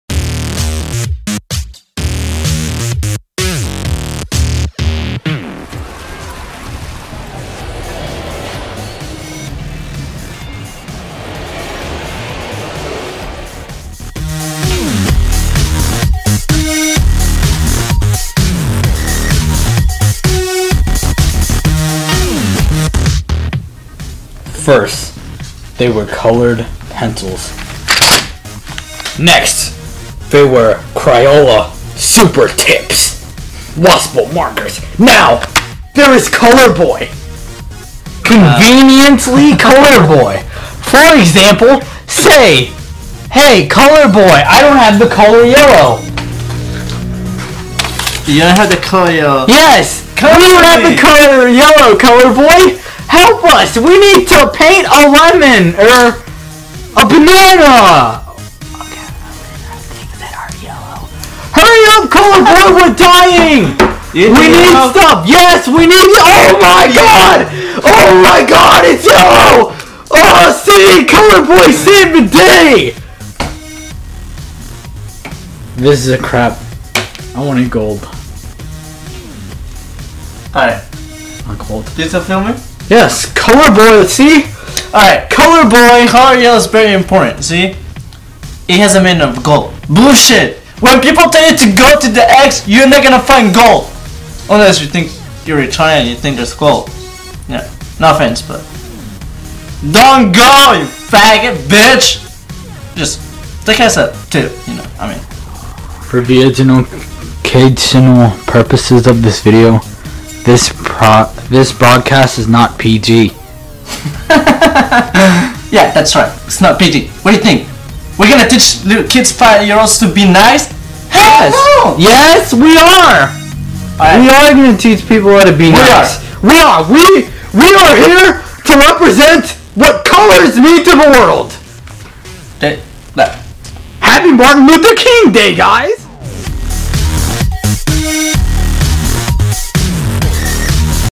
The Amazing Adventure Of ColorBoy - Colored Markers
ColorBoy Episode # 2 Showing how ColorBoy is always Prepared to Color